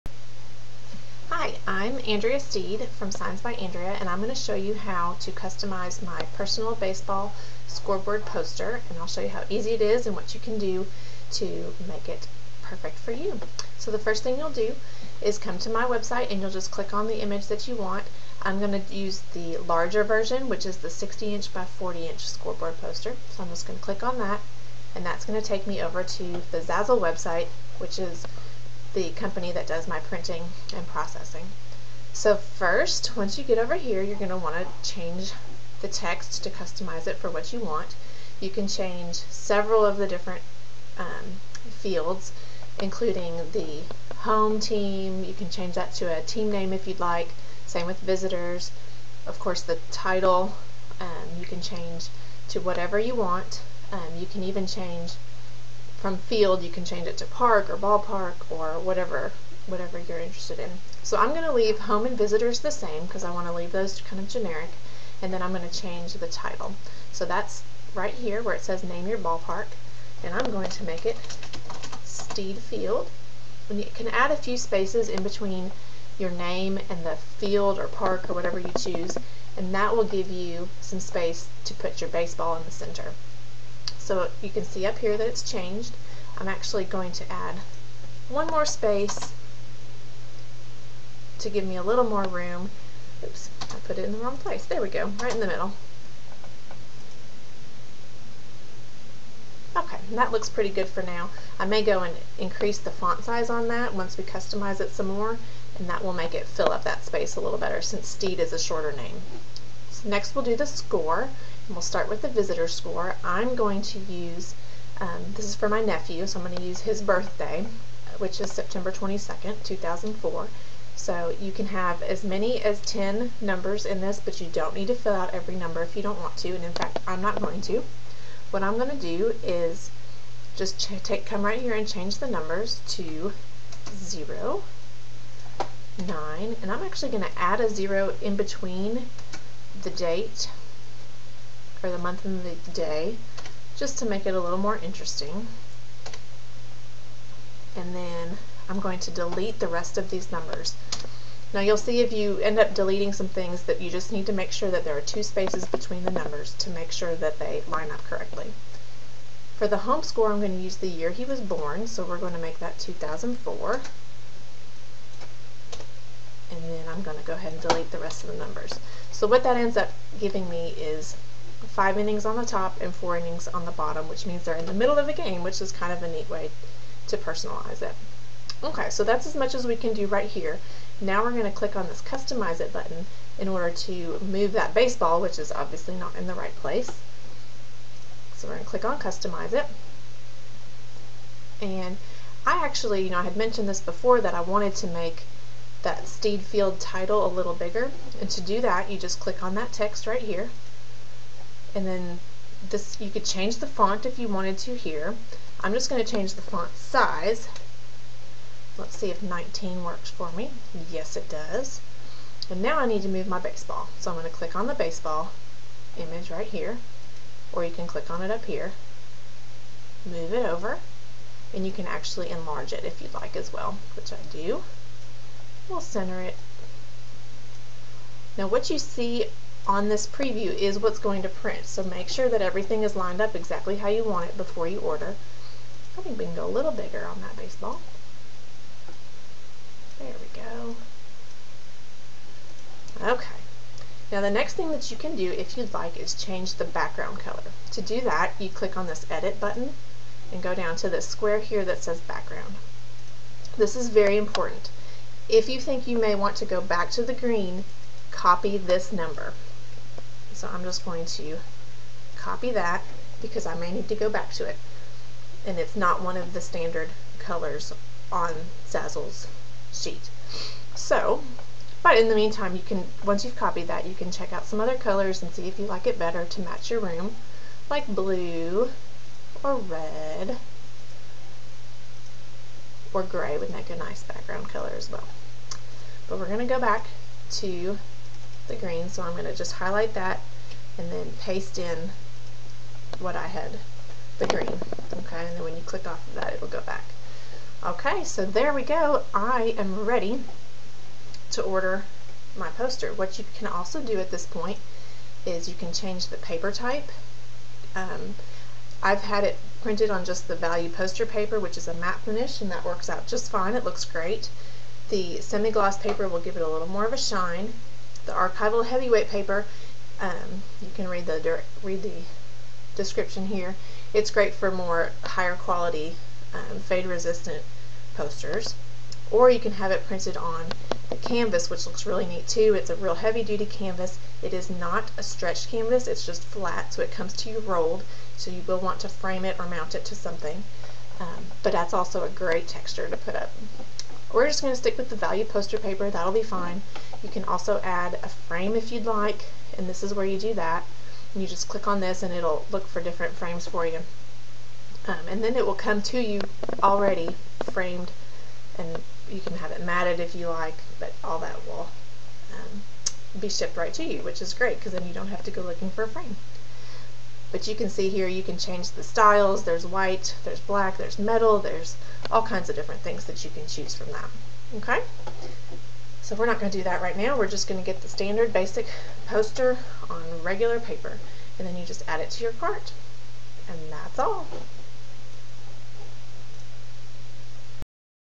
Personalizing Your Baseball Scoreboard Poster
A video tutorial showing how to customize your own Baseball Scoreboard Poster from SignsbyAndrea.com. ORDER